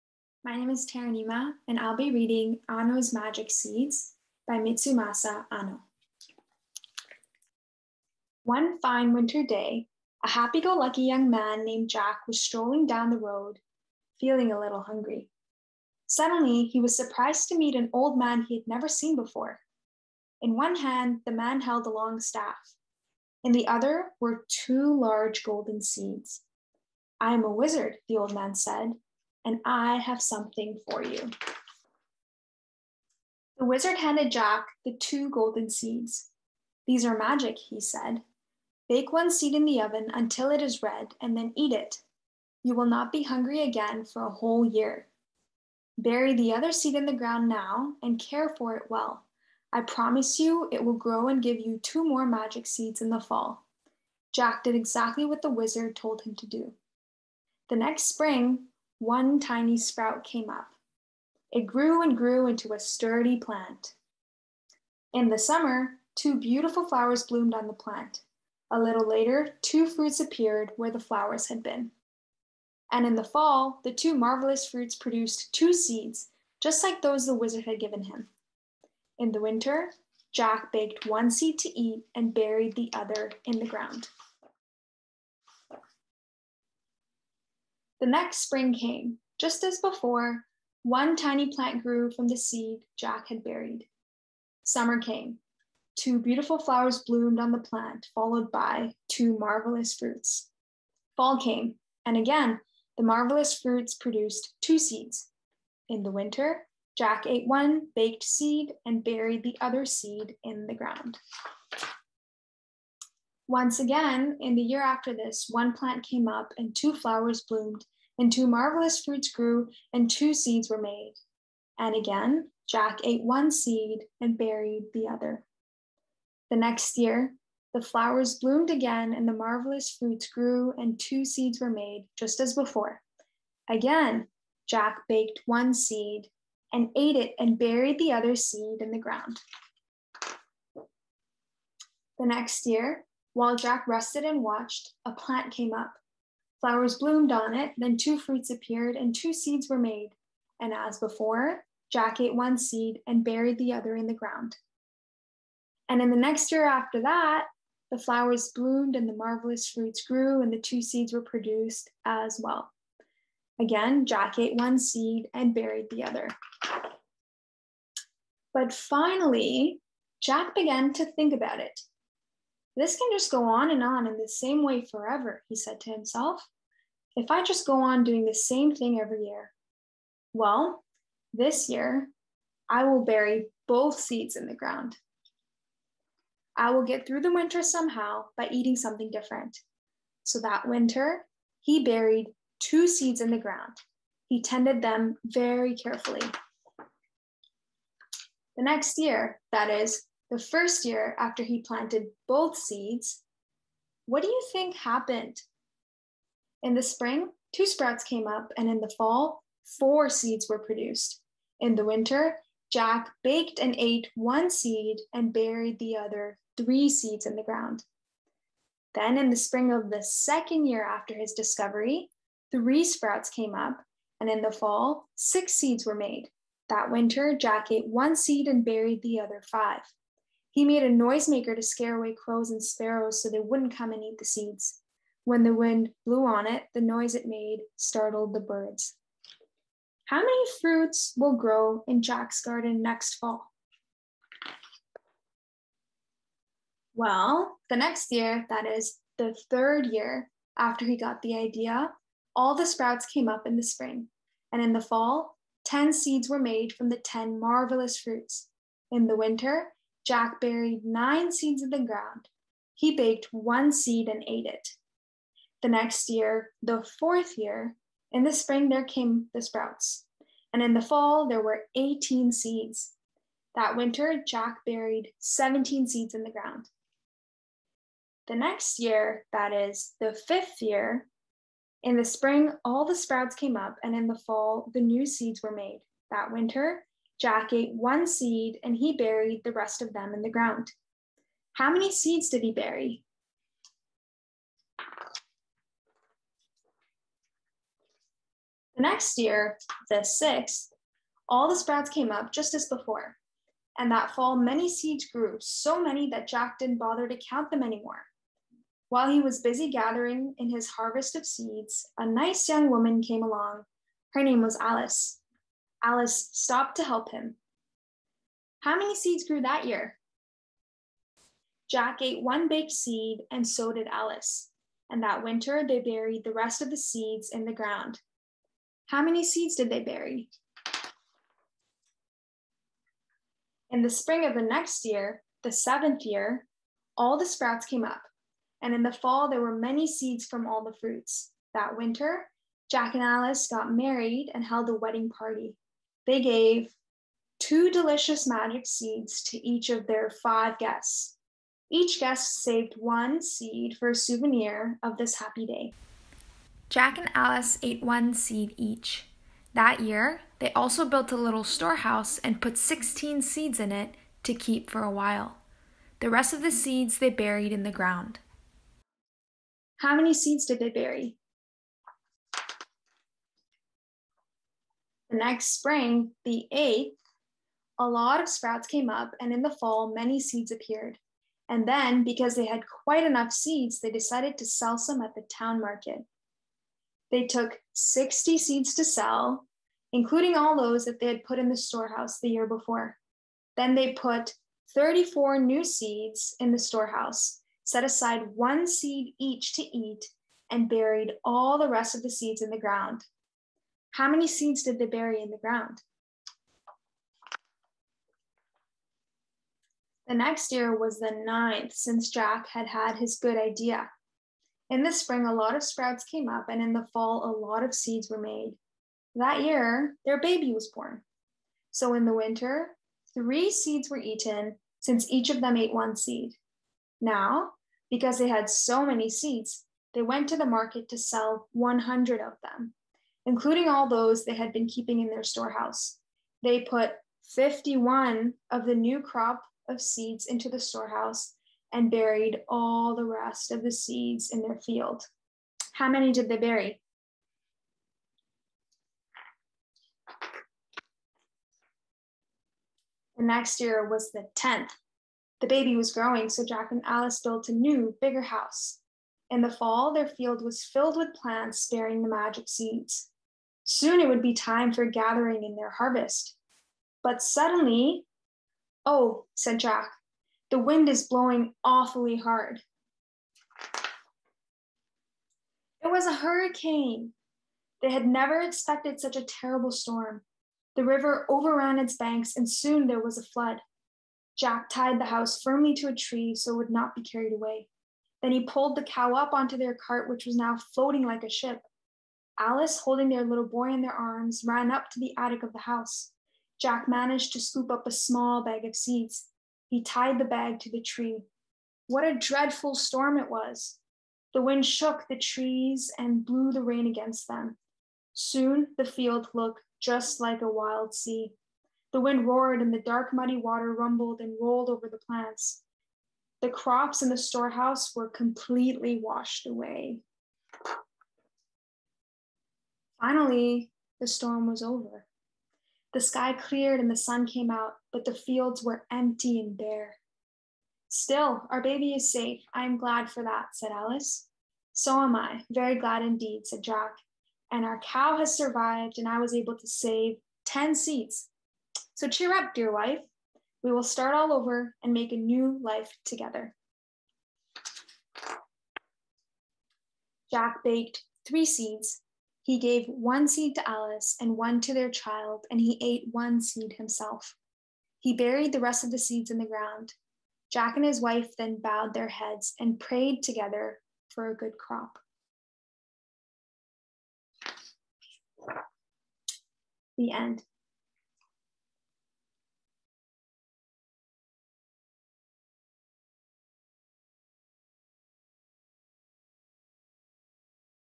Annos Magic Seeds Read Aloud: Approaching Math Through Story
The children's storybook Annos Magic Seeds touches upon a range of mathematical concepts, including simple counting to sophisticated geometric and algebraic reasoning.

Listen to Tarunima read the story and discover how you can make mathematical connections to your own class.

For a math lesson plan related to this story, along with others,